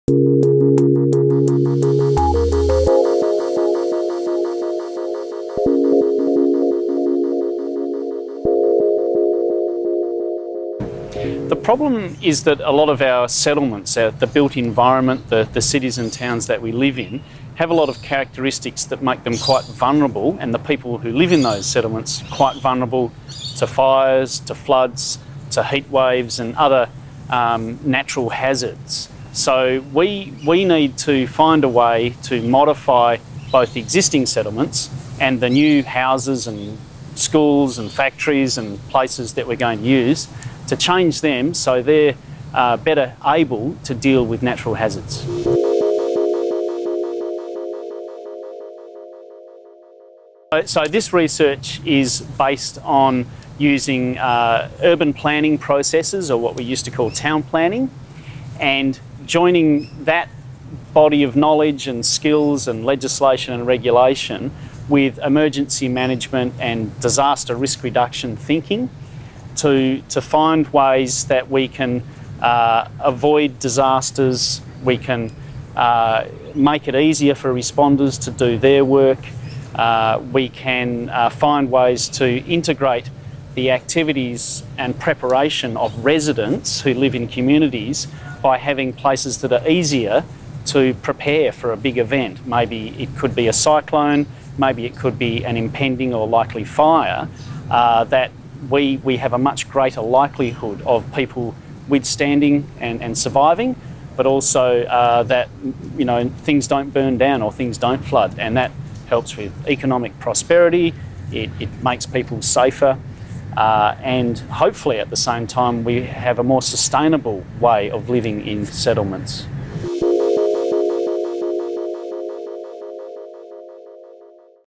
Urban planning for natural hazard mitigation - project overview
Professor Alan March talks about research that is looking at ways to ensure people in urban communities are protected against natural hazards and how it can be applied to emergency management thinking.